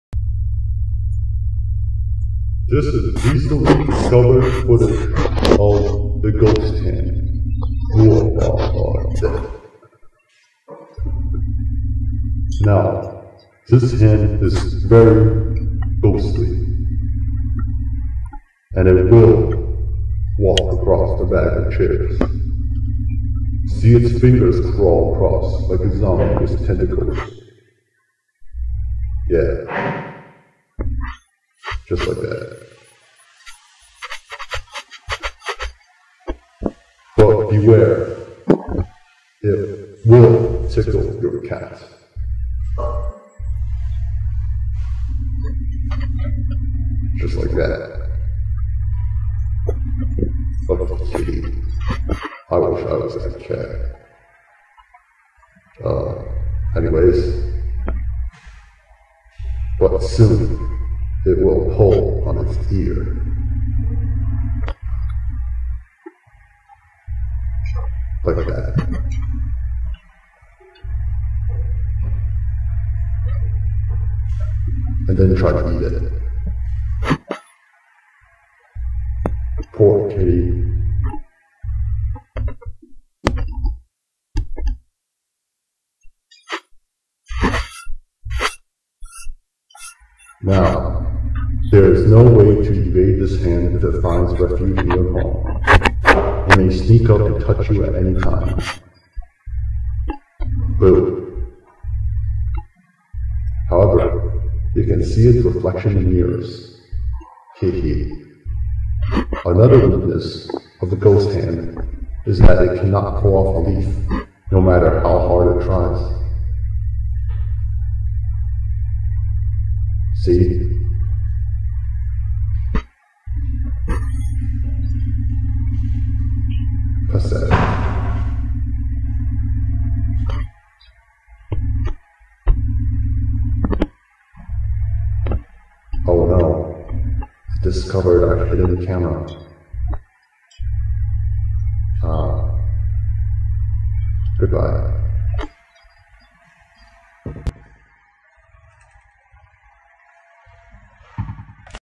My Fifth After Effects Video (The Ghost Hand)
This is my fifth After Effects video. Didn't work out so well, so yea...